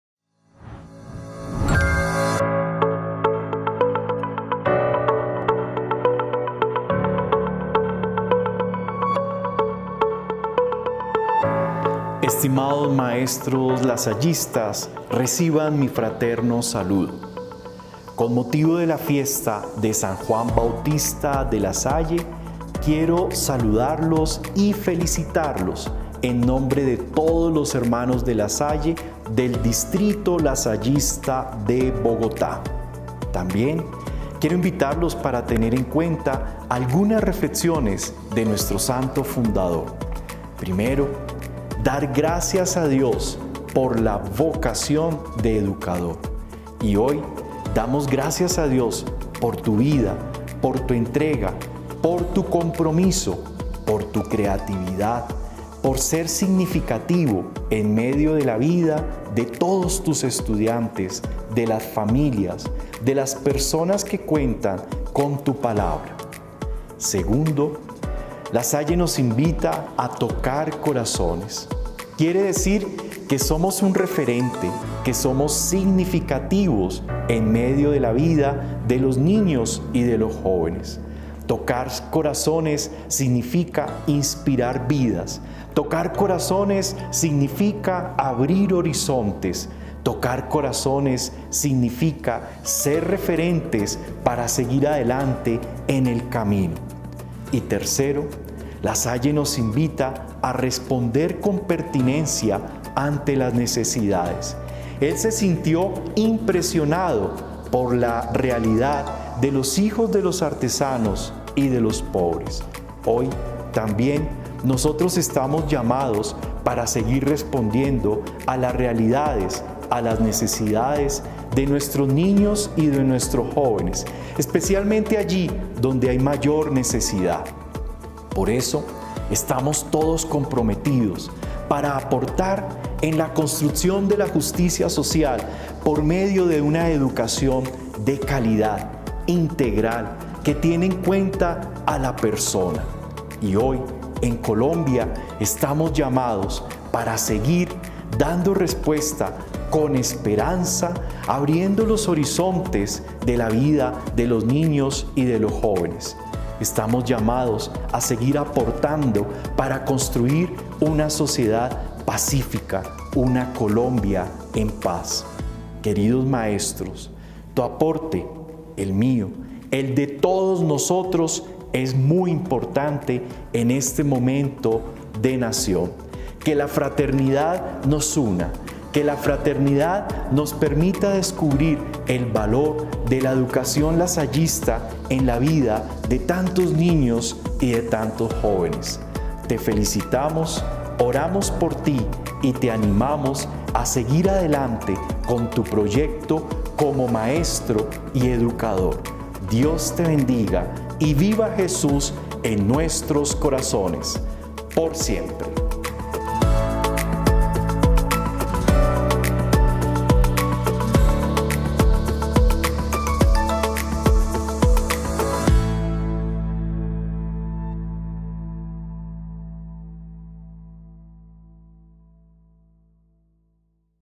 Mensaje del Hermano Visitador Diego Díaz a los Maestros
Hoy 15 de mayo el Hermano visitador Diego Díaz se dirige a los maestros lasallistas resaltando la vocación al servicio educativo de nuestros niños y jóvenes, al igual que lo hacia San Juan Bautista de La Salle.

Producción Gráfica:
Gina Jaramillo
Producción audiovisual: